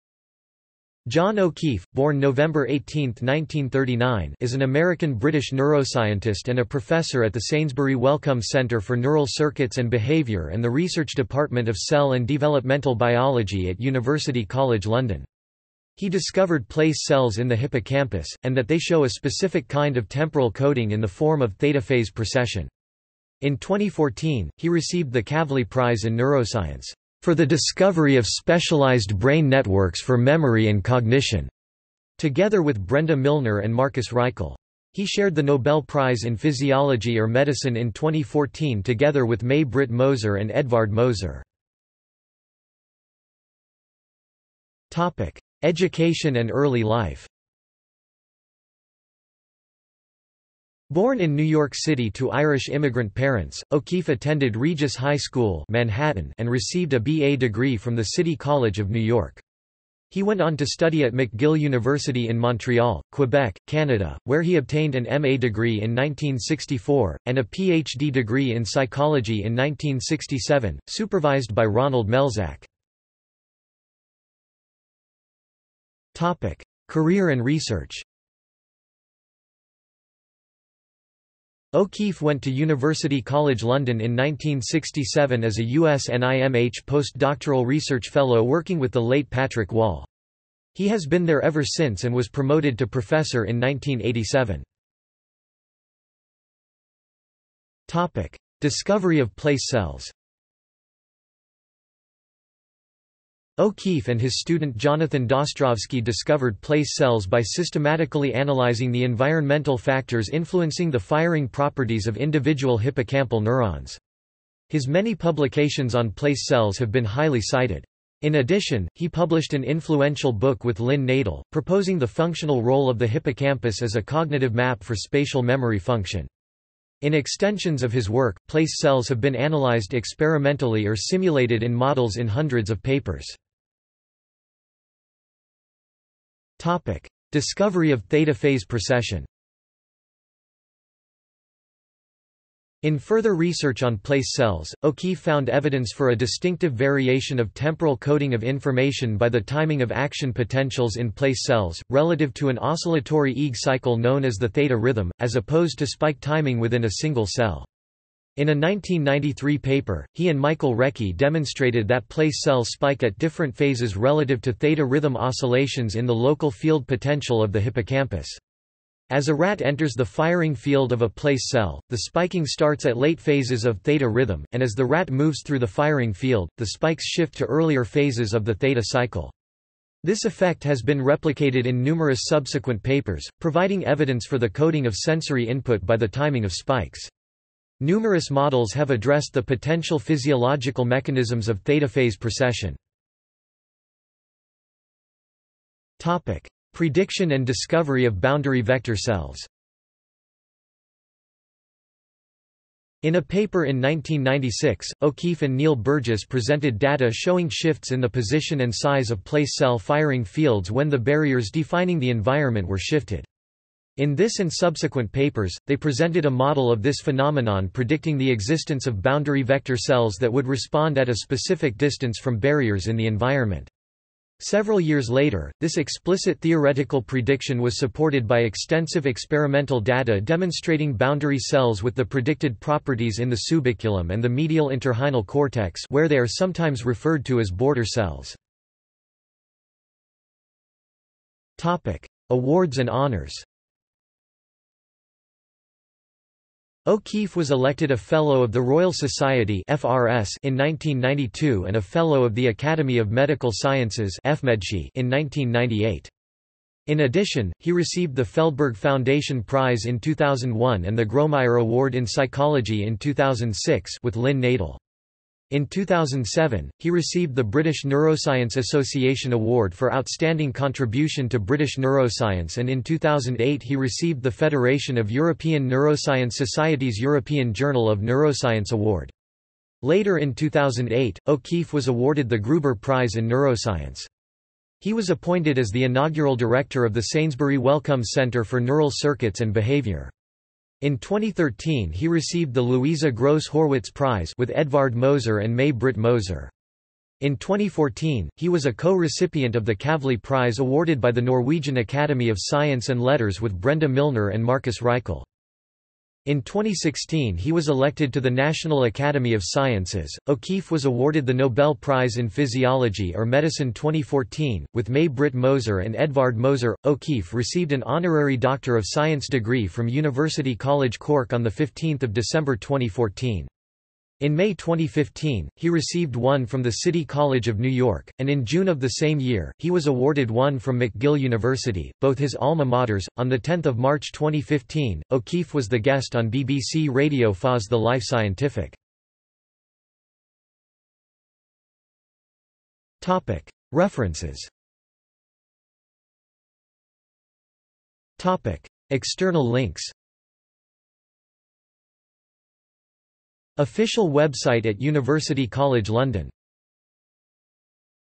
John O'Keefe (neuroscientist) | Wikipedia audio article
This is an audio version of the Wikipedia Article:
John O'Keefe (neuroscientist)

- Socrates

SUMMARY
John O'Keefe,  (born November 18, 1939) is an American-British neuroscientist and a professor at the Sainsbury Wellcome Centre for Neural Circuits and Behaviour and the Research Department of Cell and Developmental Biology at University College London. He discovered place cells in the hippocampus, and that they show a specific kind of temporal coding in the form of theta phase precession. In 2014, he received the Kavli Prize in Neuroscience "for the discovery of specialized brain networks for memory and cognition", together with Brenda Milner and Marcus Raichle. He shared the Nobel Prize in Physiology or Medicine in 2014 together with May-Britt Moser and Edvard Moser.